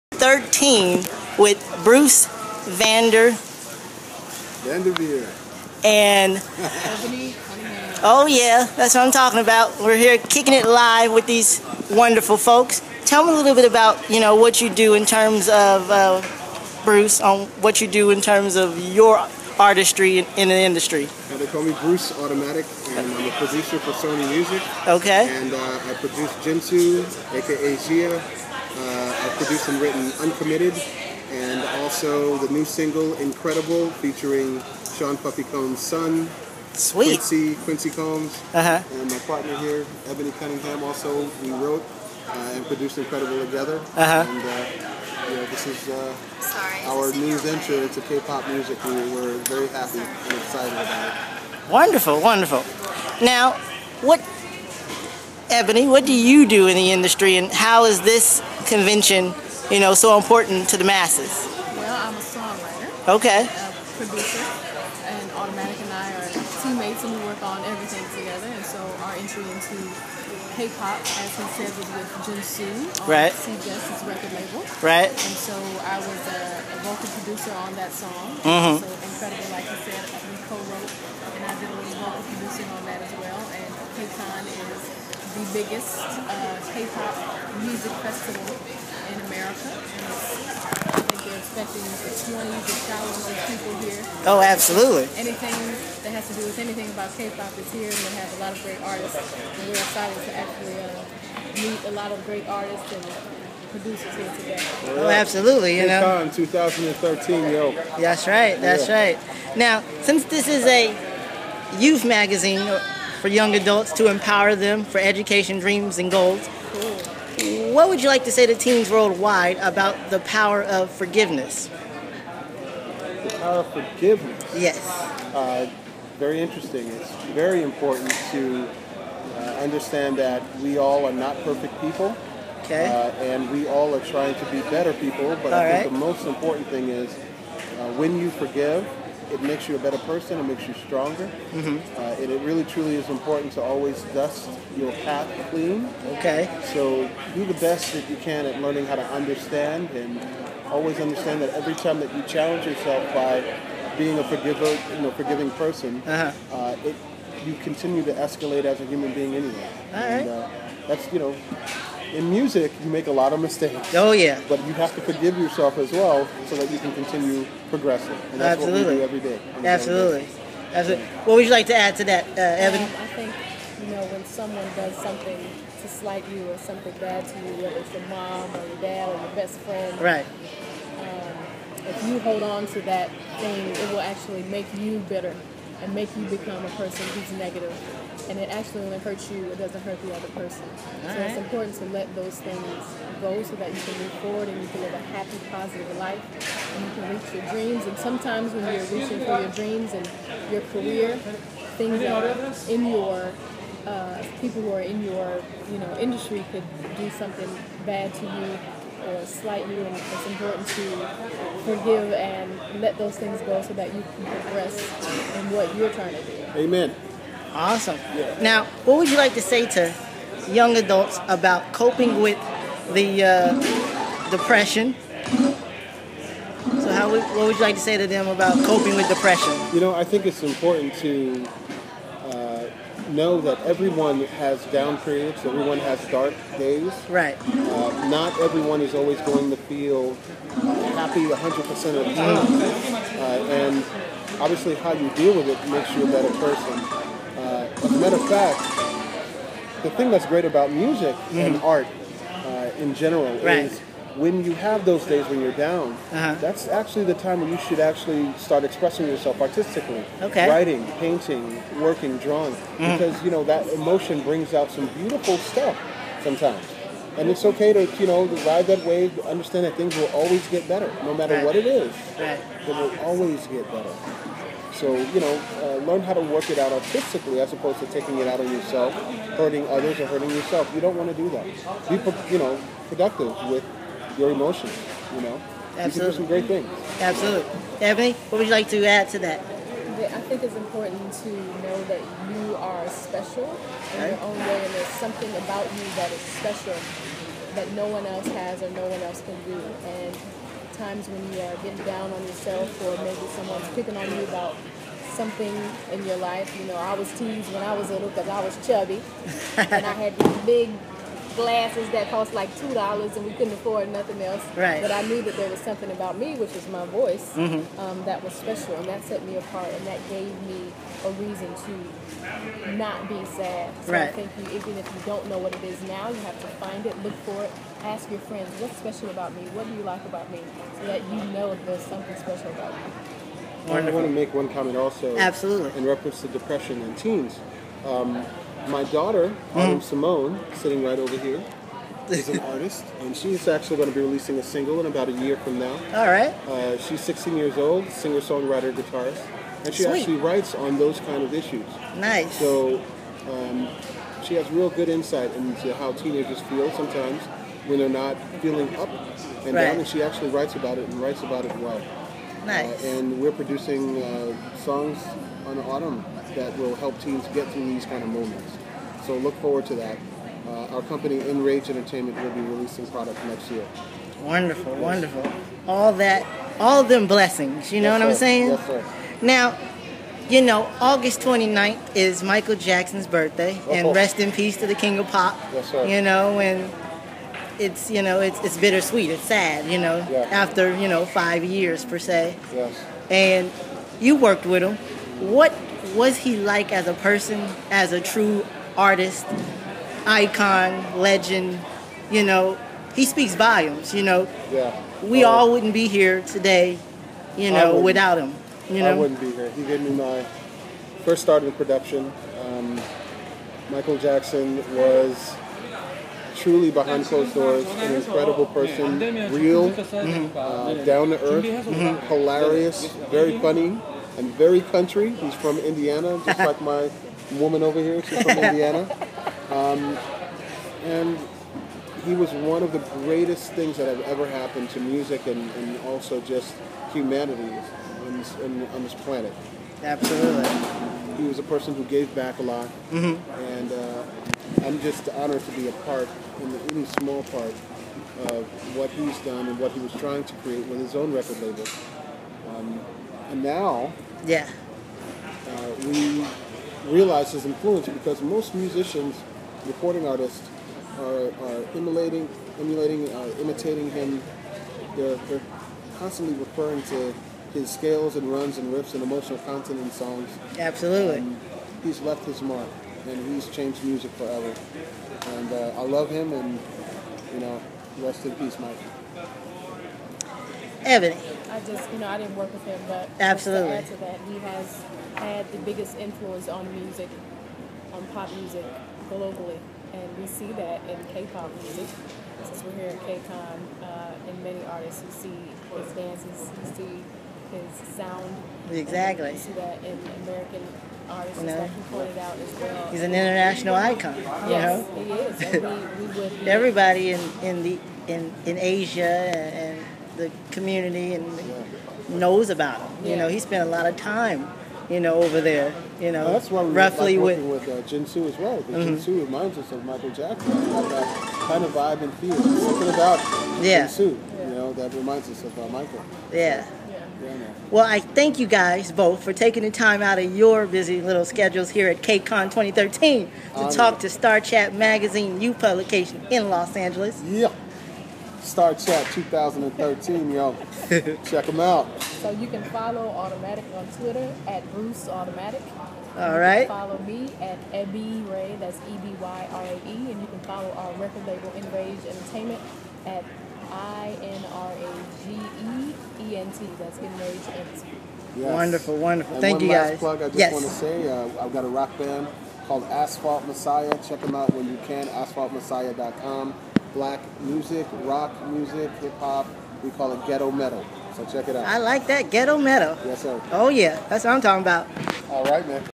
Ebony Cunningham And Bruce Automatic Vanderveer
Kcon 13 We Interviewed Ebony Cunningham A Singer Songwriter Producer
and Bruce Vanderveer  and Ebony talk about working with Junsa (XIA)
and uplift the youth in the process